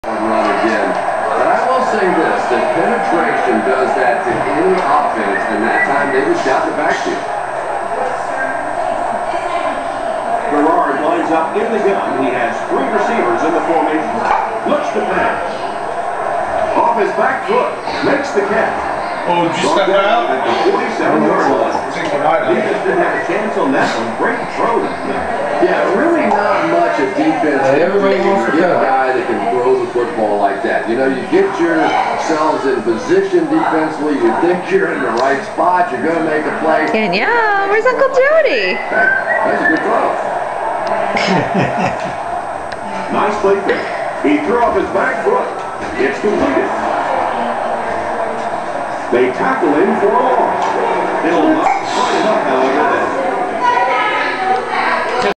Danielle and the Melnick Boys
Danielle took a liking to Uncle Jody's leg.